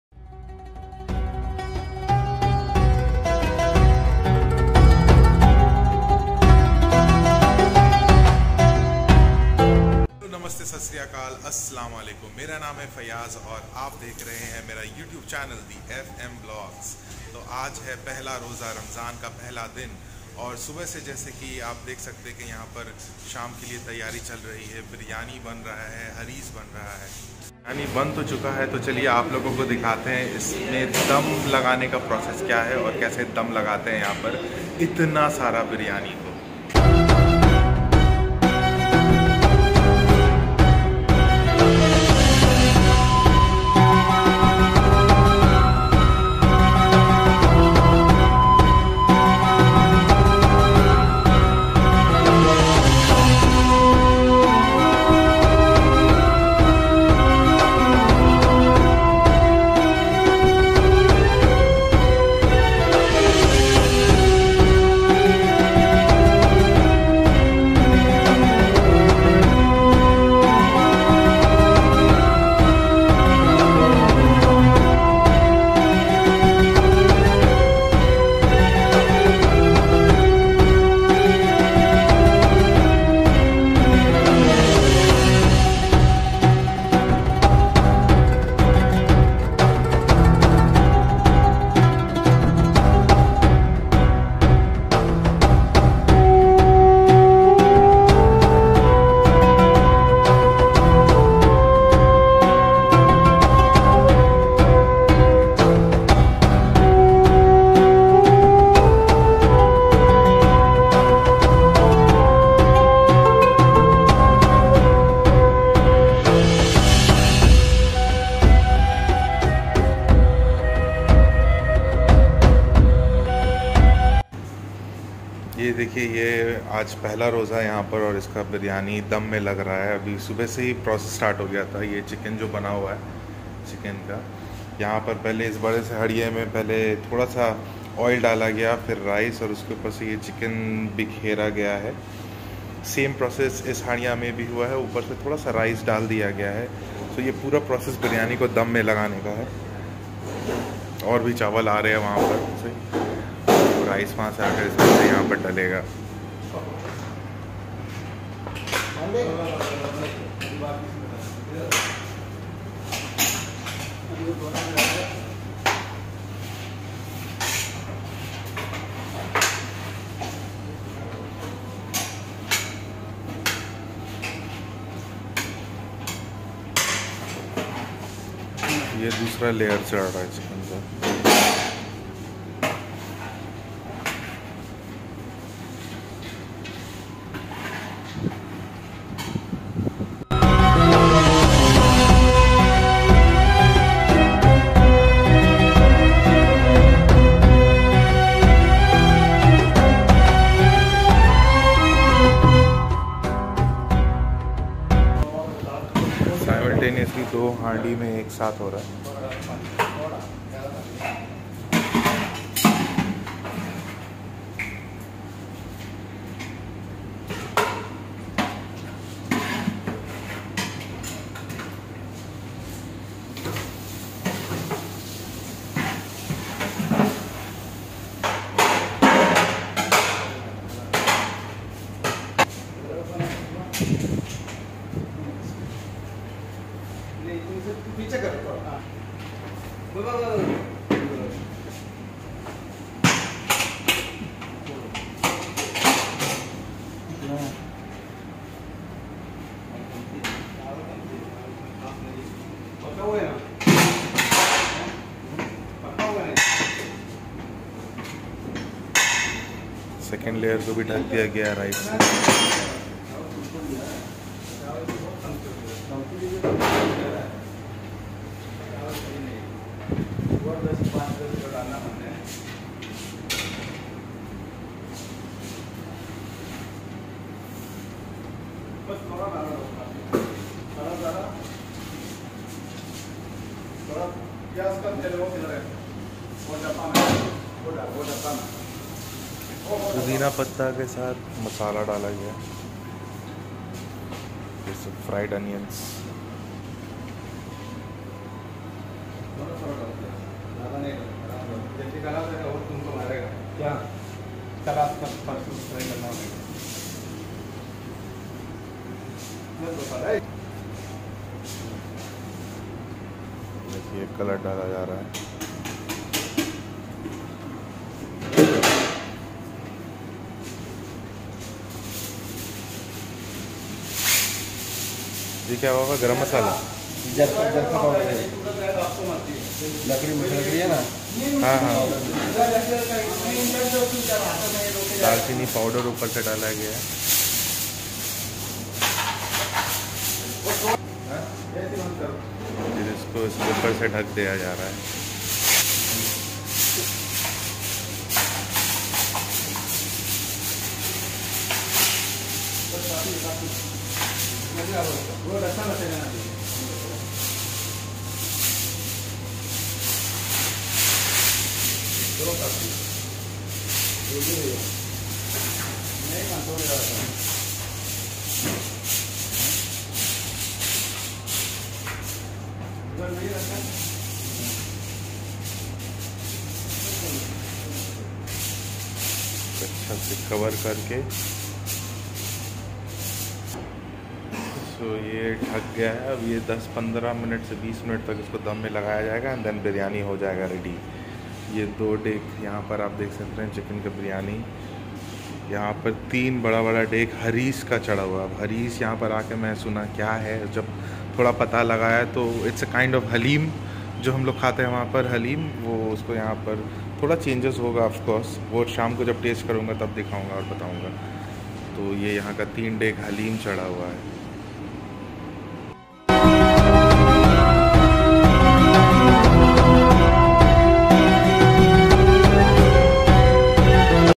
40 KGs Chicken Dum Biryani | Arabic Style | FM VLOGS
The content shows how 40 kgs of Chicken Biryani is prepared for the Ramzan distribution...
Treat to experience.